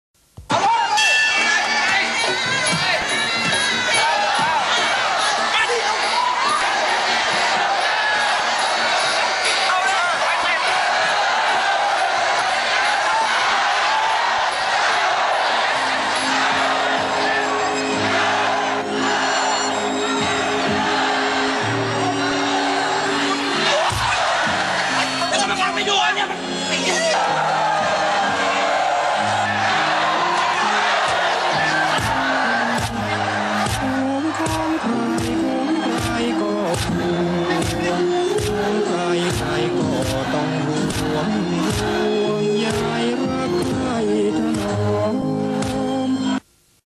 Thai Boxing Viagra
Funny Thai Commercial for Viagra.